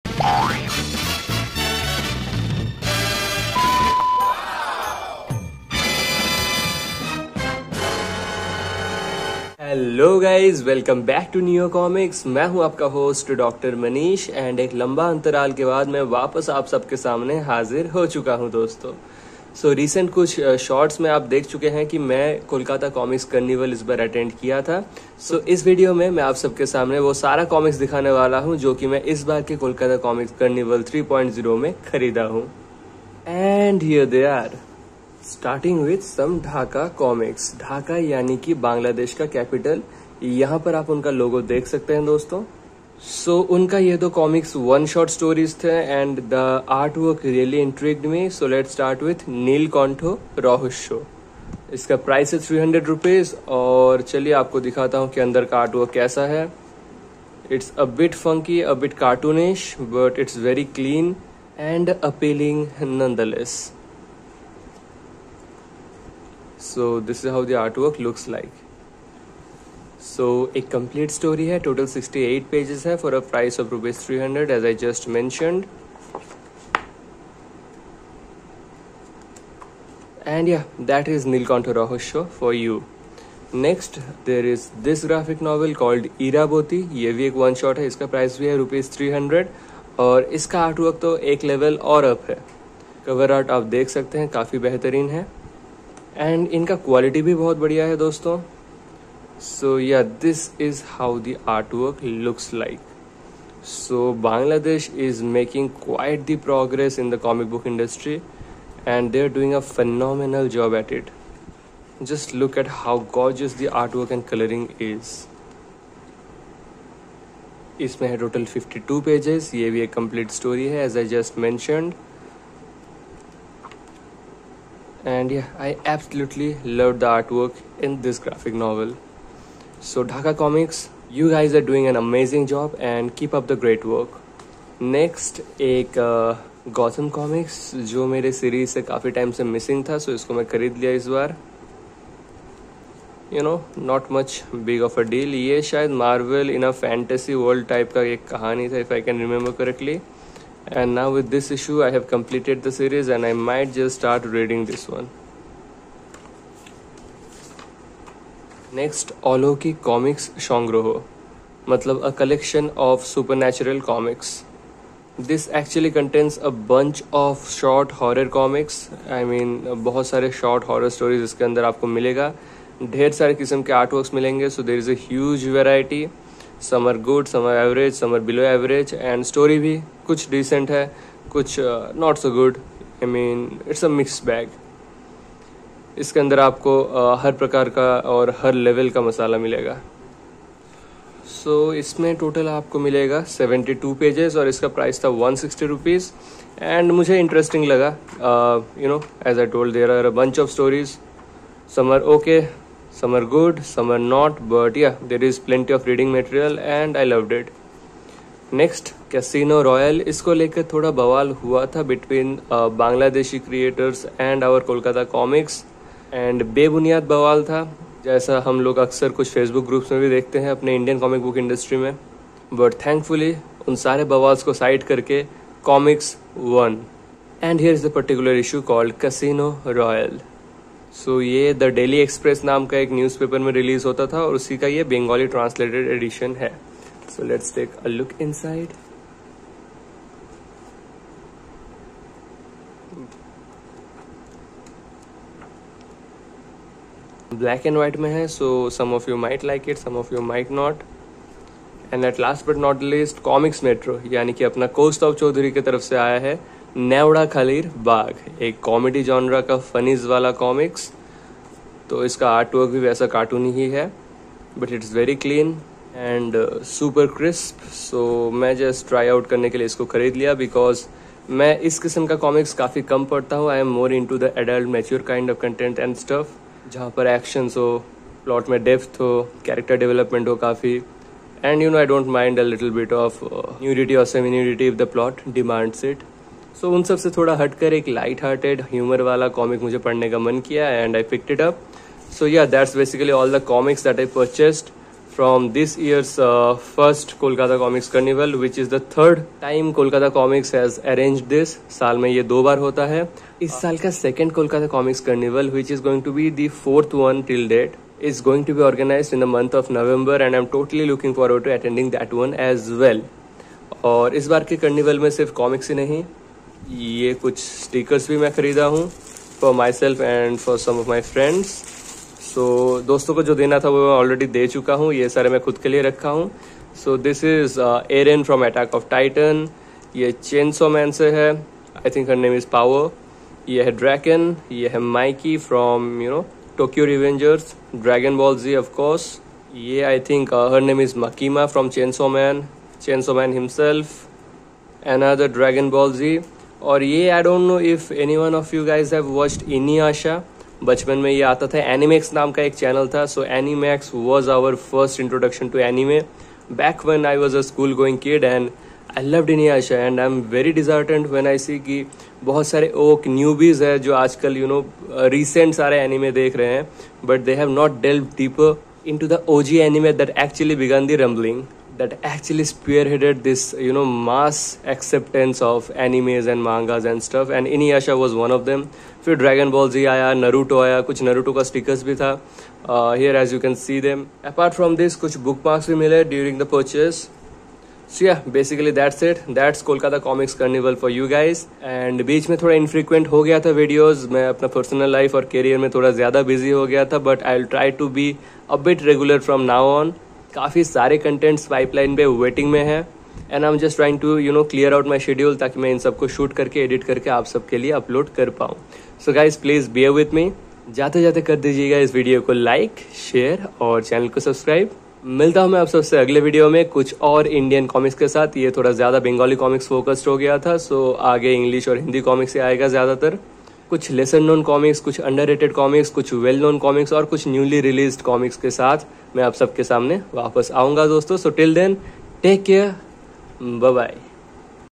Comic Book Haul from Kolkata Komics Karnival 3.0! Dhaka Comics, Comics Metro, Anime stickers & more!
Bangladeshi Comics are leveling up and setting new standards for quality comics... have a look at some of the Bangladeshi Comics (@dhakacomics8525) I purchased recently. There are a bunch of other comics and A
cool vinyl anime stickers included in this video aa well... have fun watching it!